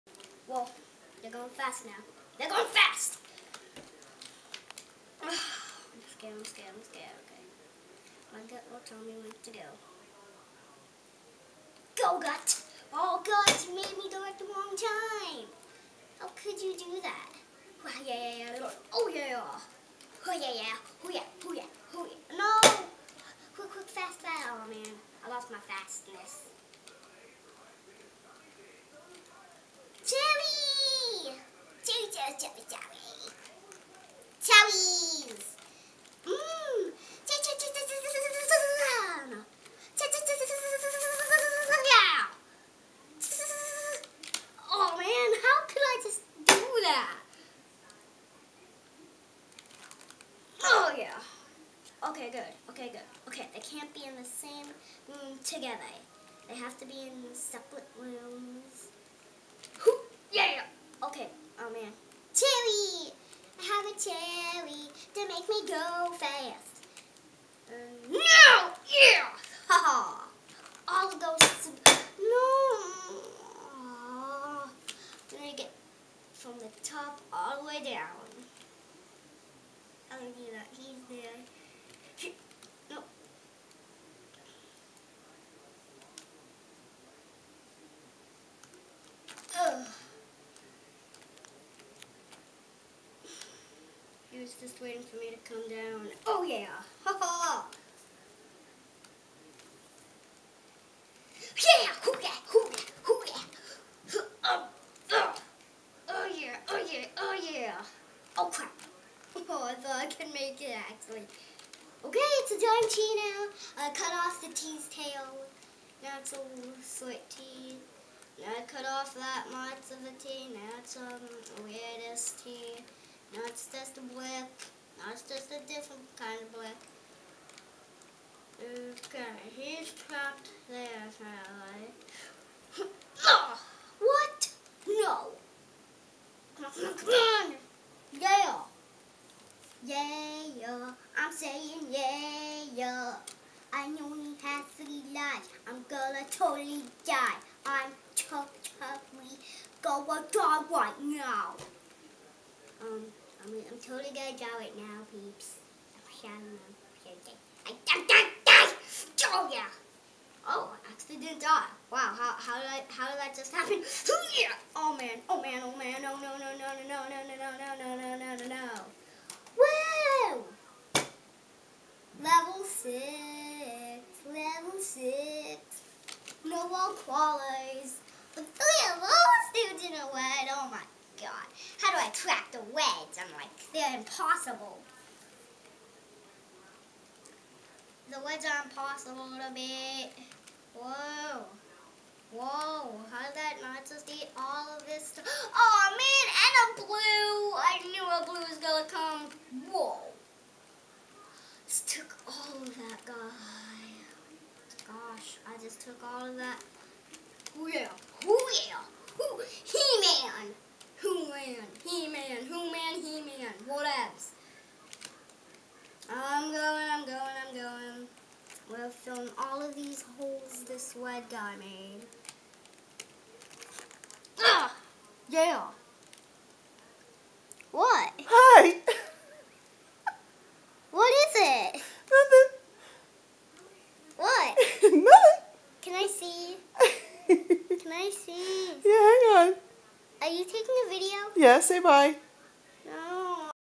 Max Playing a PC Game
Mom's stealth cam strikes again. Max was cracking me up with his inner dialog (which was totally out-loud - he is wearing headphones and can't hear himself talking.)